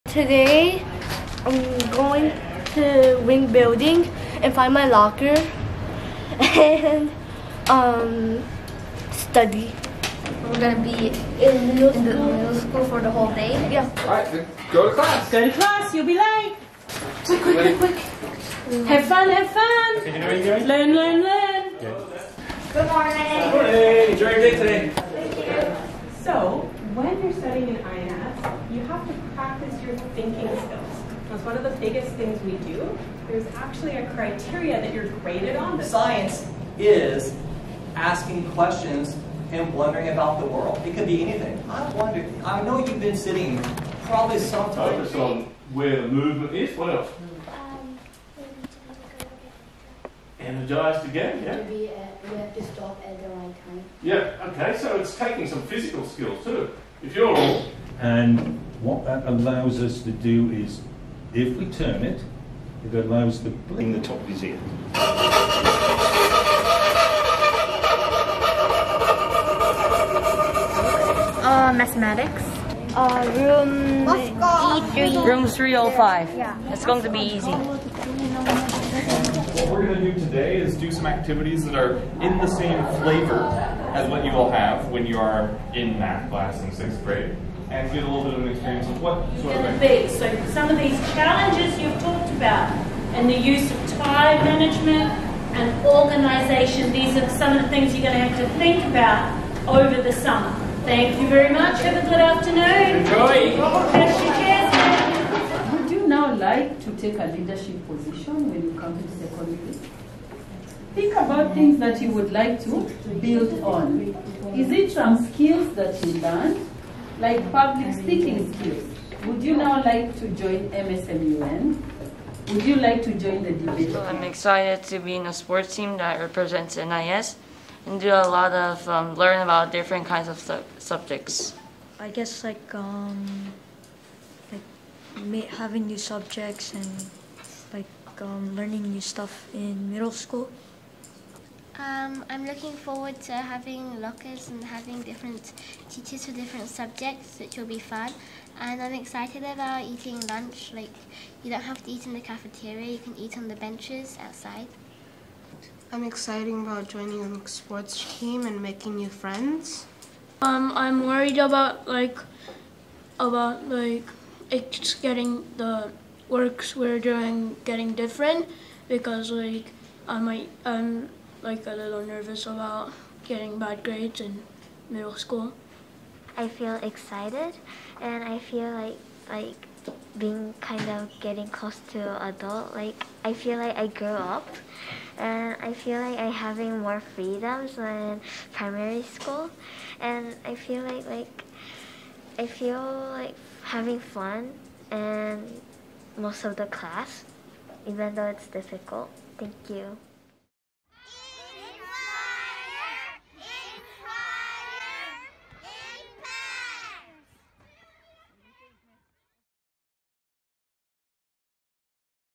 Getting ready for MYP
Gr. 5 students moved up to Gr. 6!
They got to live the life of a Middle School student, spending the whole day going to middle school classes and meeting teachers.